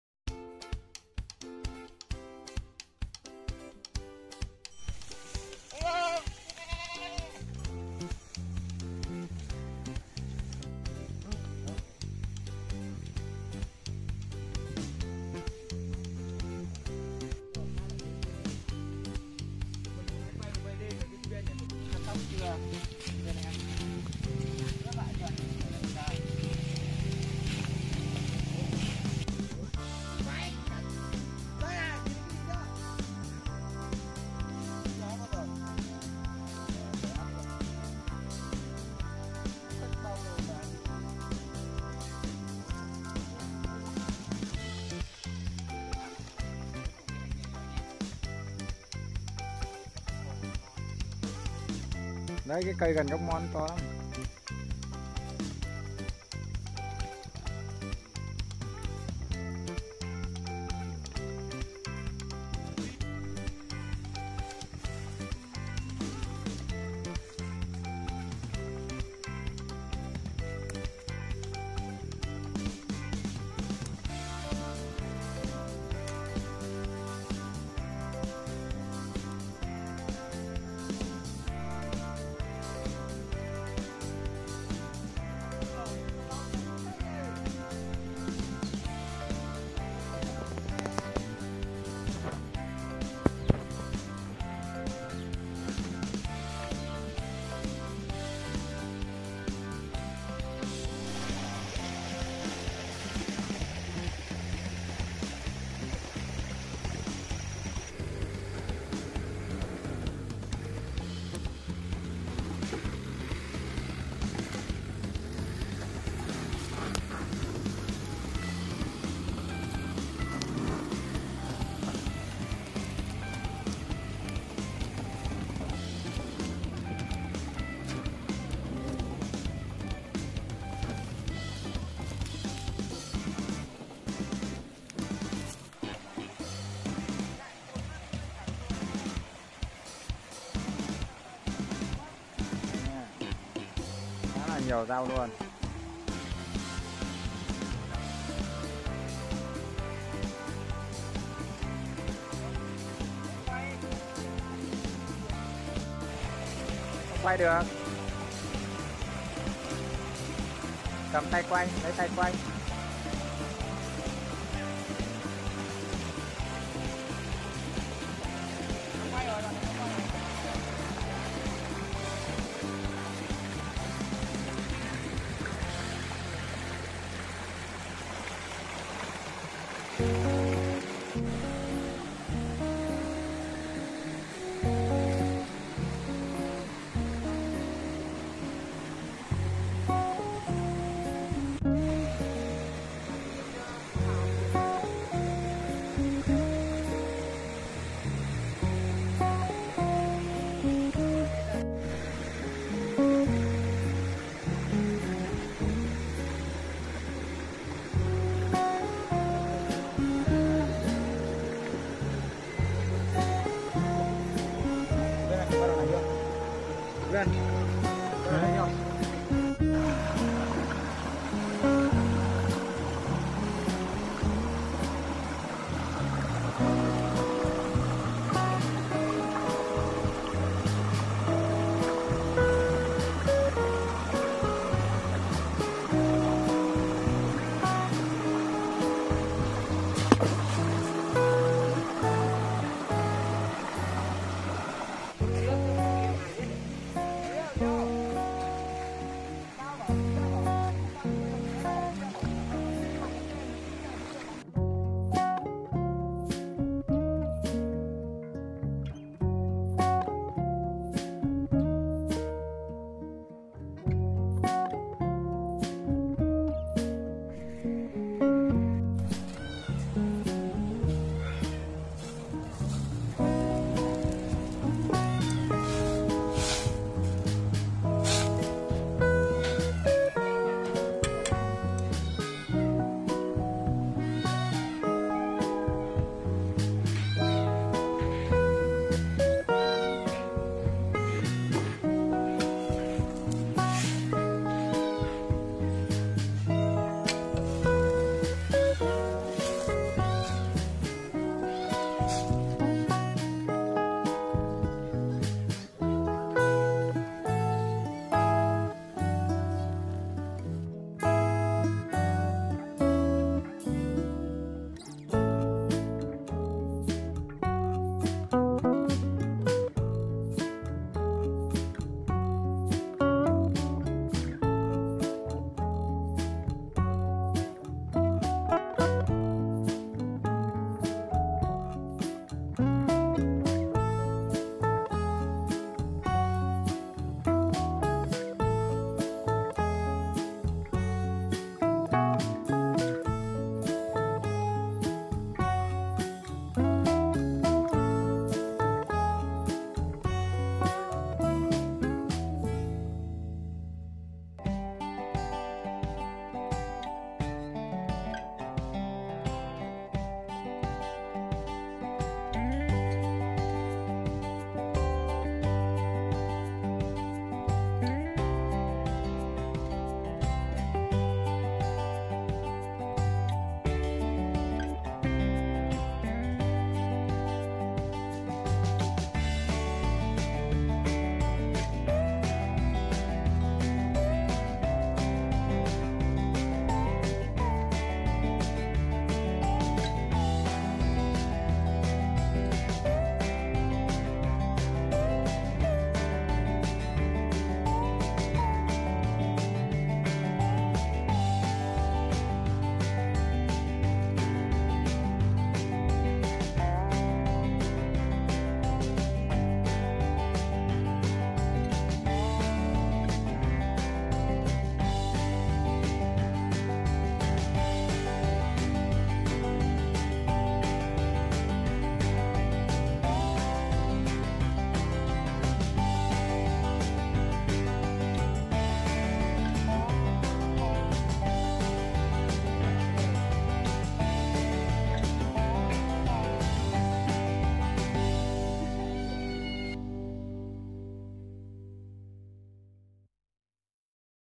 How to make a delicious forest vegetable salad. cách nộm rau rớn rừng ngon tại nhà
cách lấy và nộm rau rừng ngon